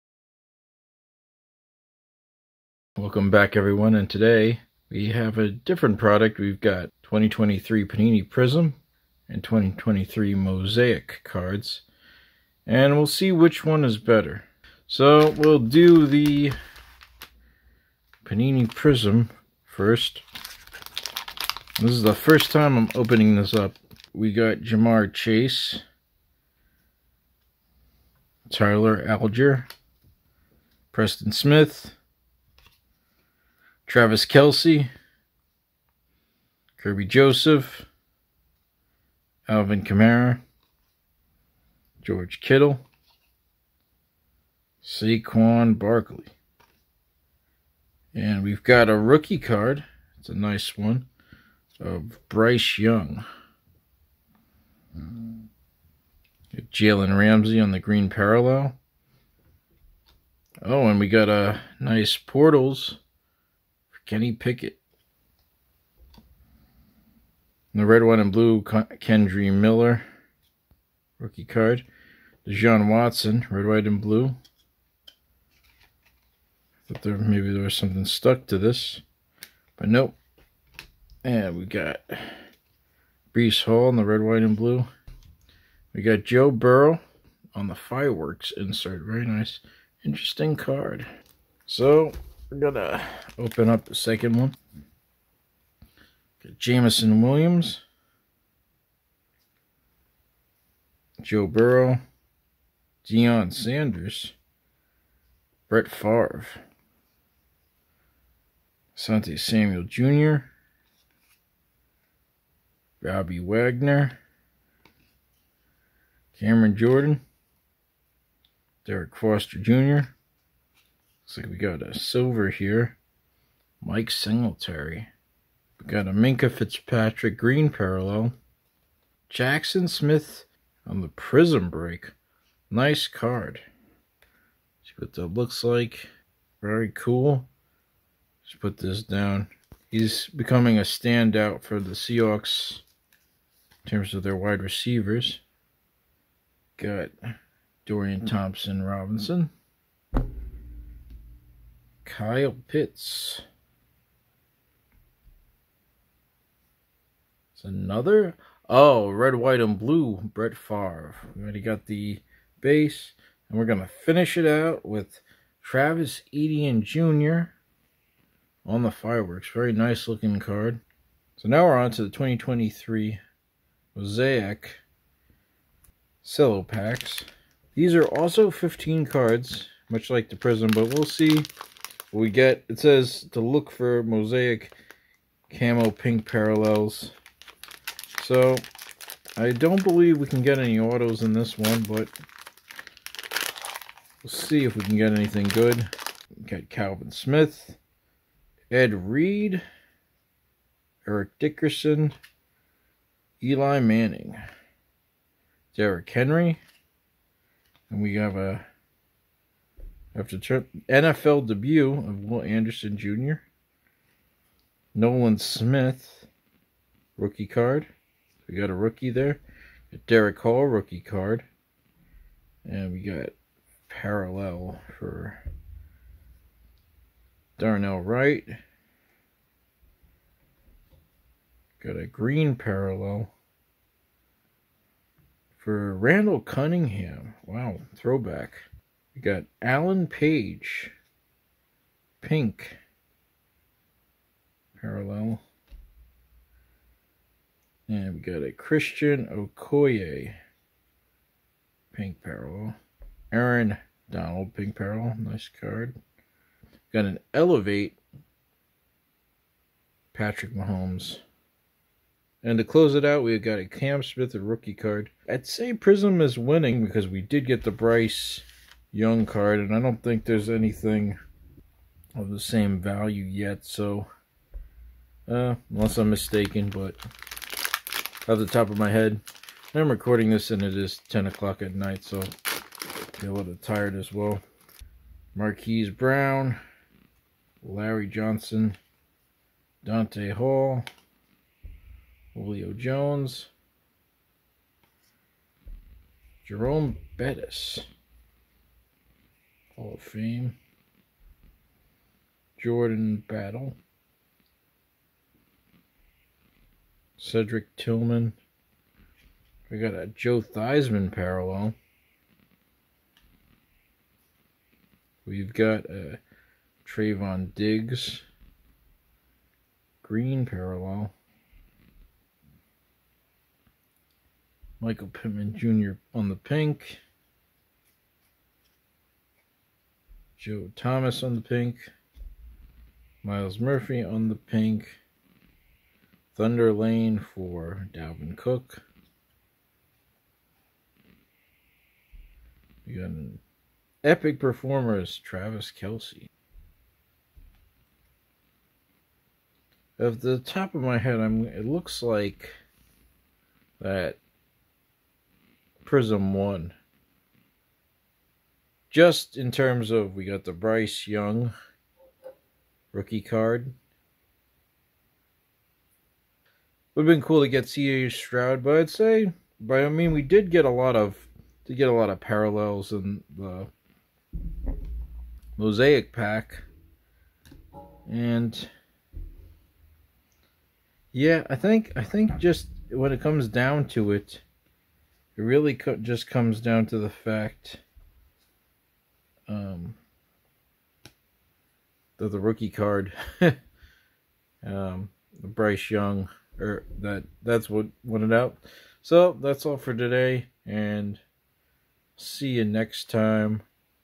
BRYCE YOUNG SILVER PRIZM HIT! 🏈 Prizm Vs Mosaic Value Pack Battle.
See who will win between 2023 Prism and 2023 Mosaic Value Packs. Same card amount. Same Price. One winner.